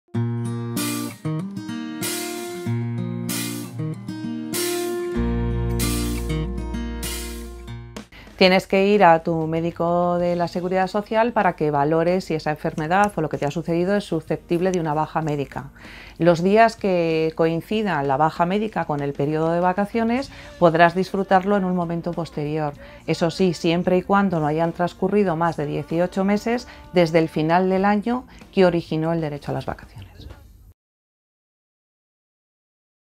¿Qué ocurre si, estando de vacaciones, tengo un accidente o caigo enferma/o? - CONSULTAS LABORALES
¿Estando de vacaciones te han tenido que dar la baja por alguna circunstancia y no sabes cómo gestionar eso con tu empresa? Pilar Caballero, abogada de CCOO Servicios, responde a una de vuestras dudas.